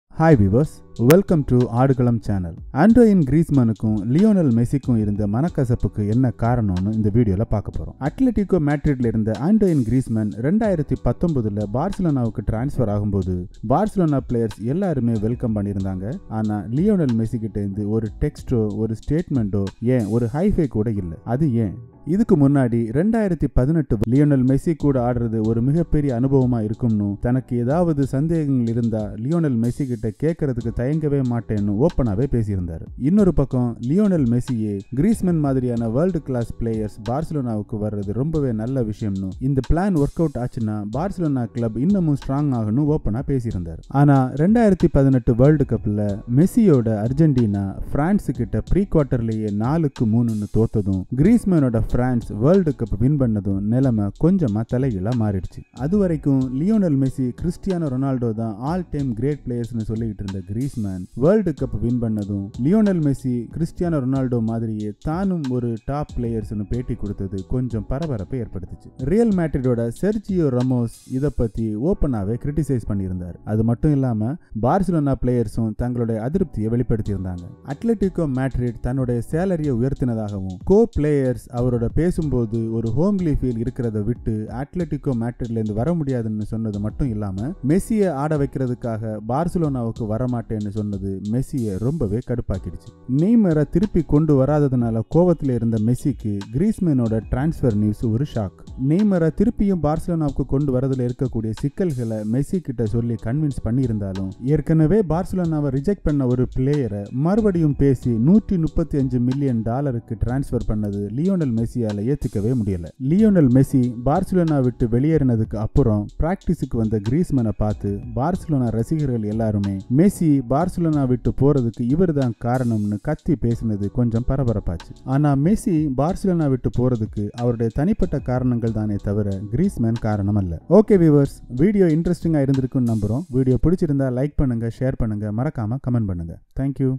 மெஸ்ஸி கிரீஸ்மேனை வெறுத்தாரா ?? | Messi | Griezmann | Football Tamil | Barcelona
this video explains why messi was unsatisfied when Antoine griezmann joining Barcelona